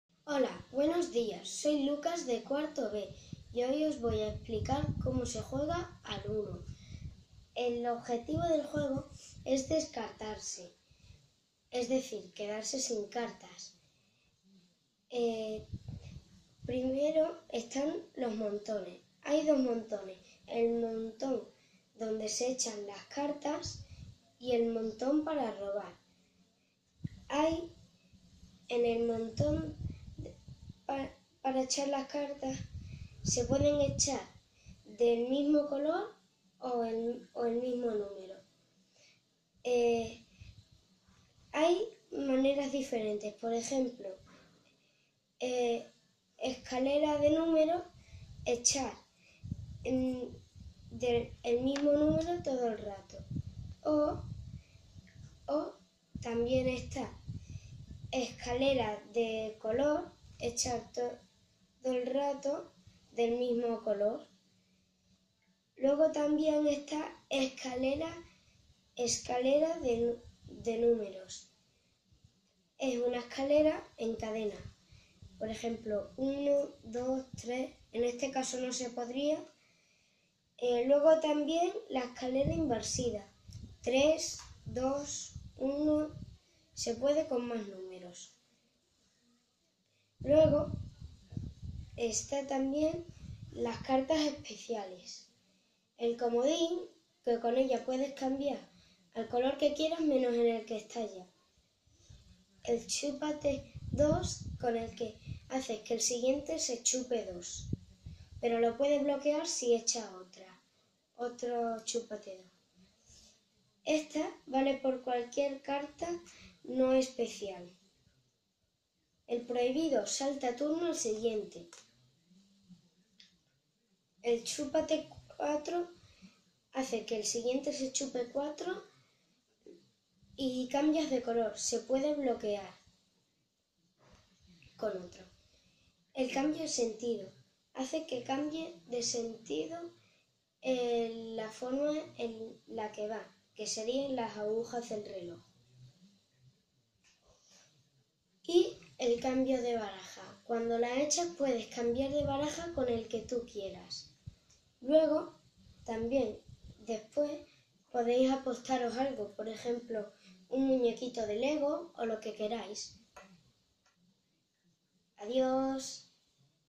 Cómo jugar al UNO - Lucas Silveira - P4B - CEIP Alba Plata
Instrucciones de cómo jugar al UNO.